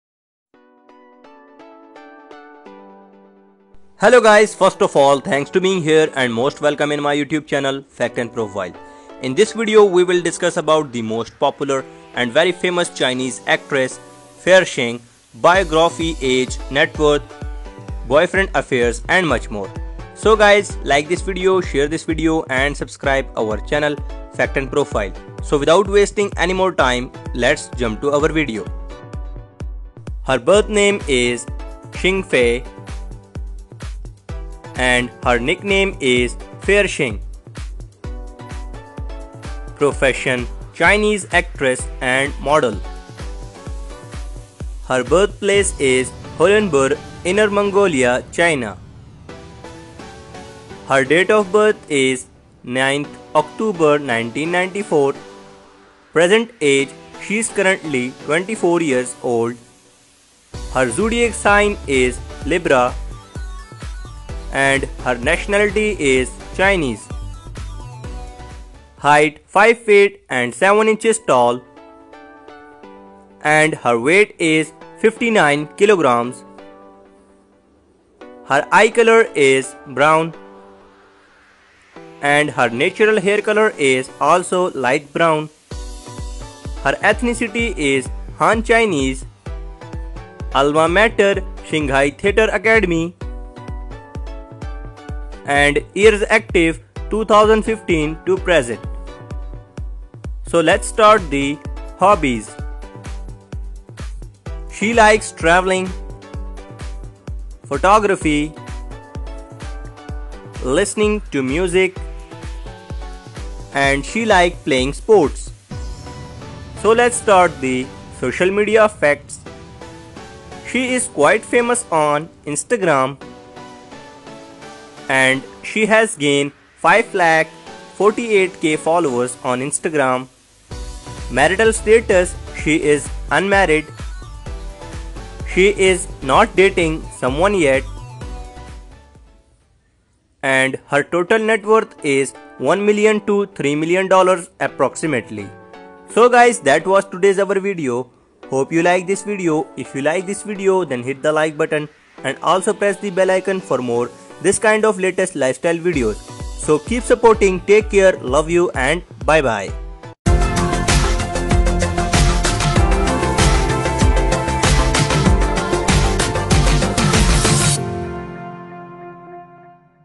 Xing Fei (Forget You Remember Love) Lifestyle, Networth, Age, Boyfriend, Facts, Hobbies, & More...
The Most Popular And Most Beautiful Chinese Actress Xing Fei Lifestyle Video 2020.